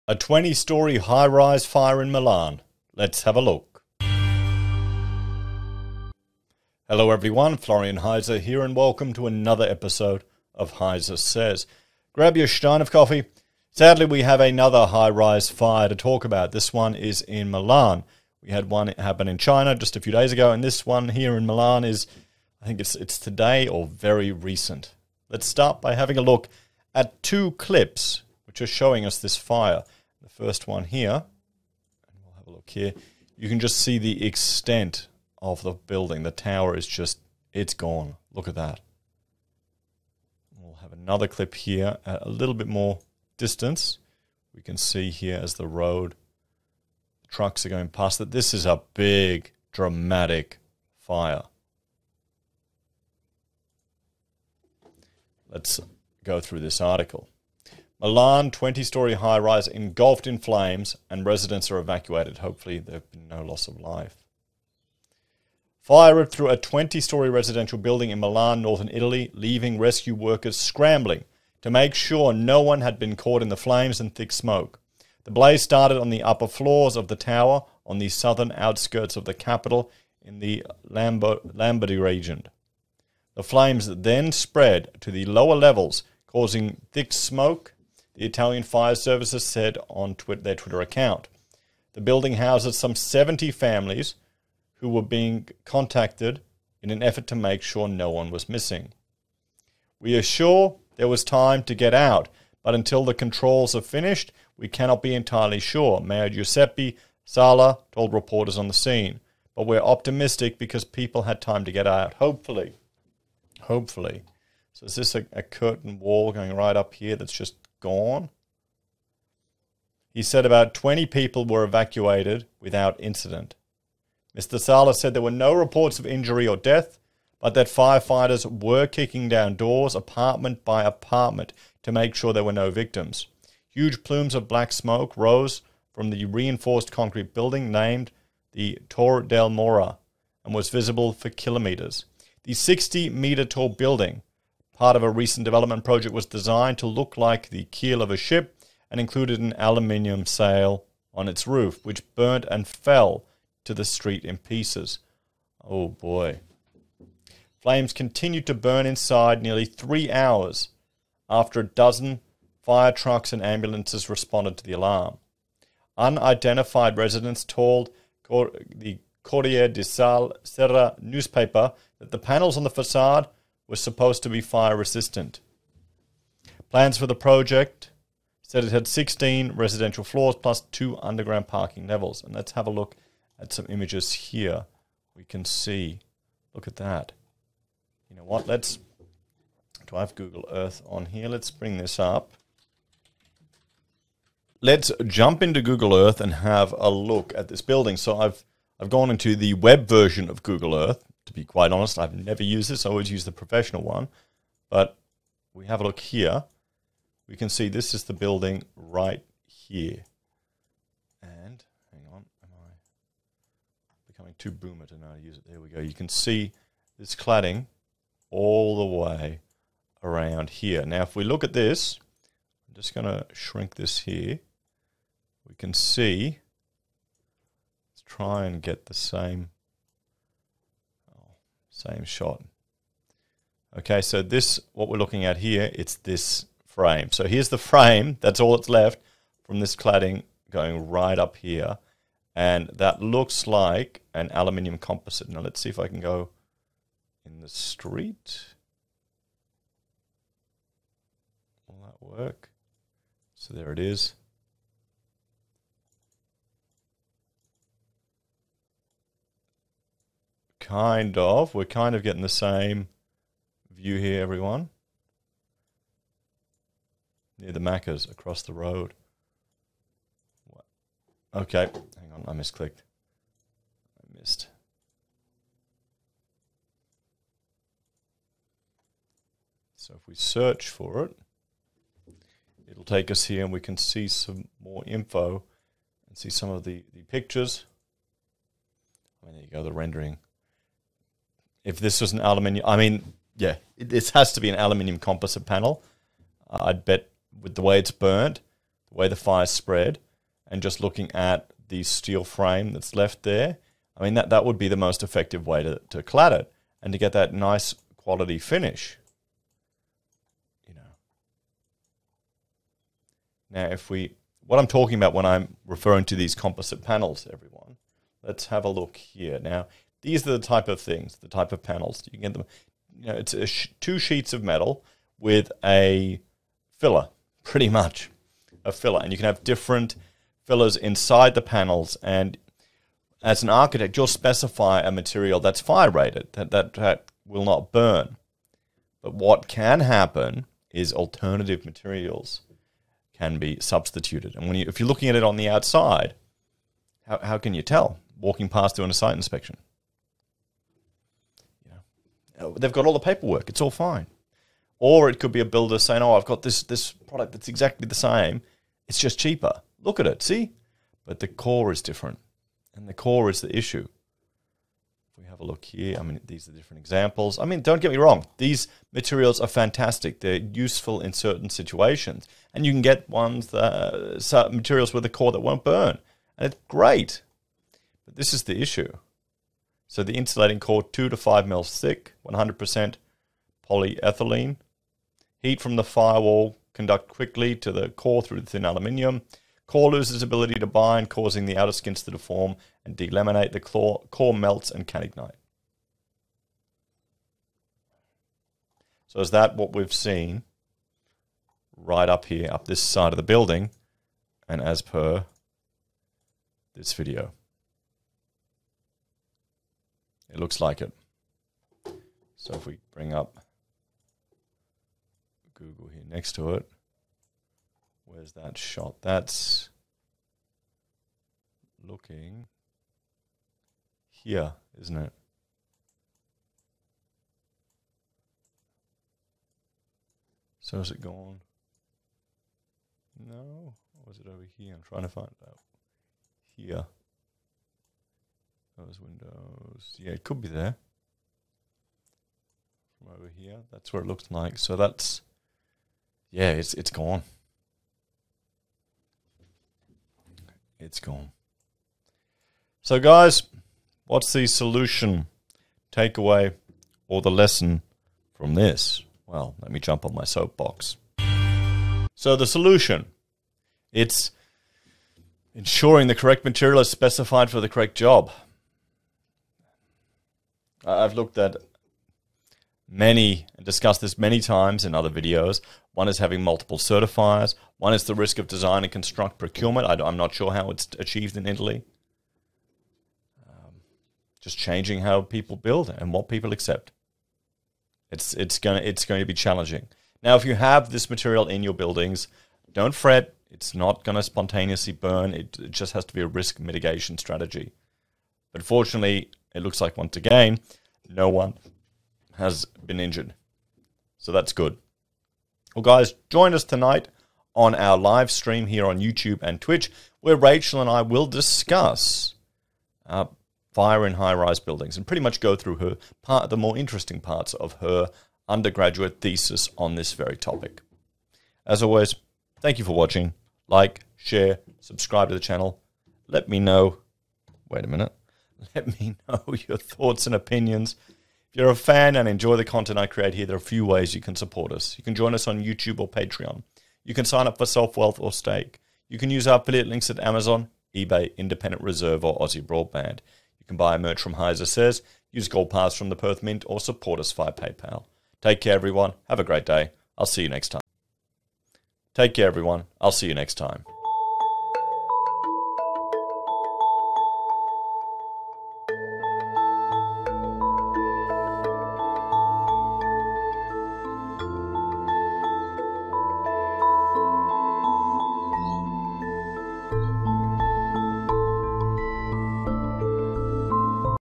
20 Storey Cladding Fire In Milan
A Twenty storey cladding fire in Milan. Apparently, the cladding was not flammable.

Why Buildings Stand Up
Why Buildings Fall Down Why Structures Fail
Great Streets
A Pattern Language

Heise Architecture
Our Architectural Practice.